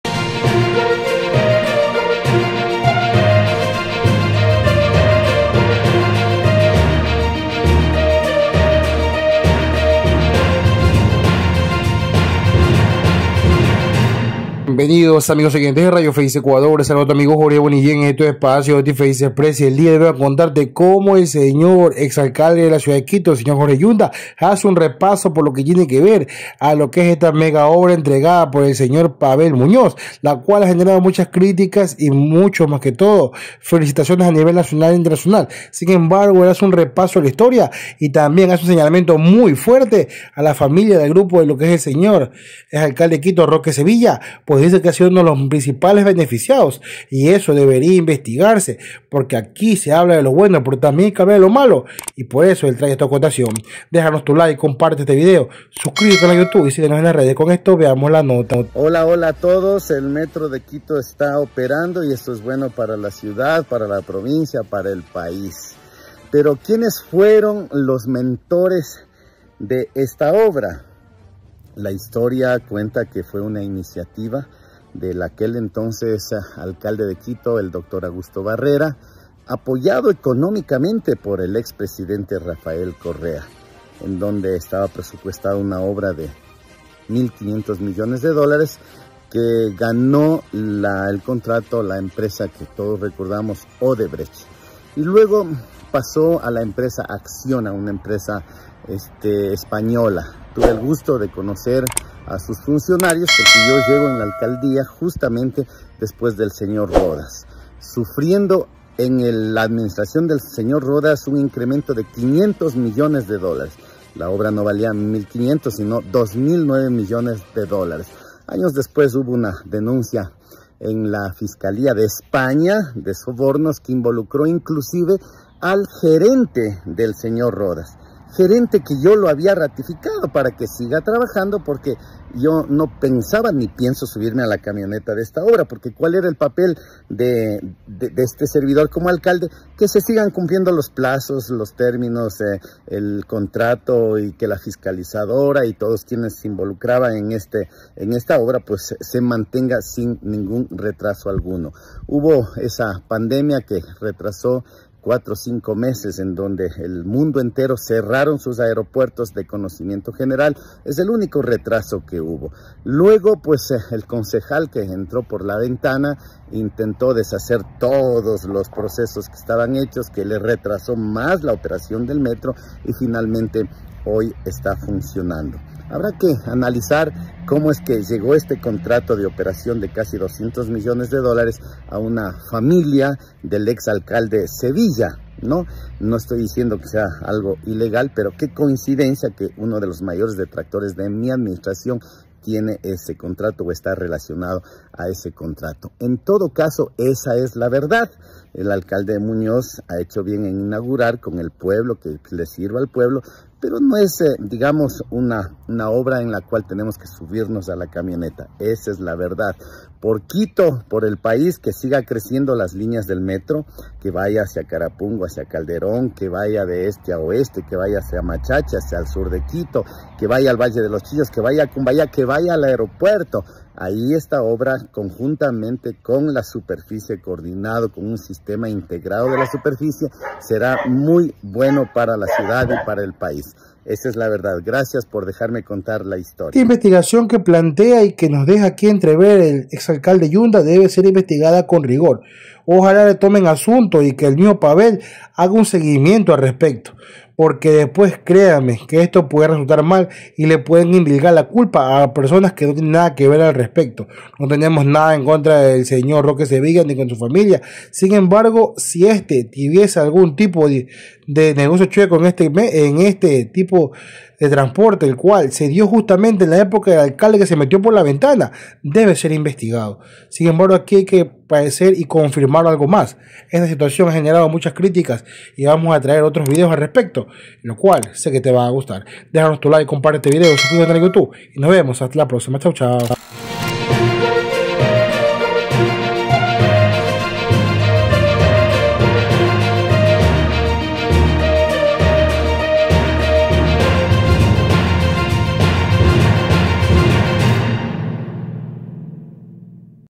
JORGE YUNDA REVELA POSIBLE CASO DE COR.RUPCION DE ROQUE SEVILLA EN EL METRO NOTICIAS RFE TV
EX ALCALDE DE QUITO JORGE YUNDA HACE TREMENDA REVELACIÓN DEL POSIBLE CASO DE COR.RUPCION DE OYTO EX ALCALDE DE LA CAPITAL EN EL METRO DE QUITO NOTICIAS RFE TV 👇